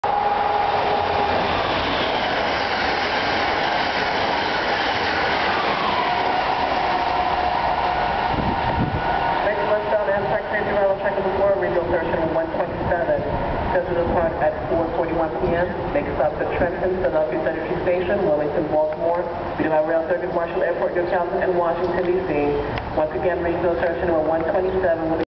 Acela Express 2125
blowing past Metropark Station on the express track at 90 mph heading to Philadelphia, Wilmington, Baltimore, BWI Airport & Washington D.C. at 4:36 p.m. as an annoucement for Amtrak Regional #127 (which I caught) comes on.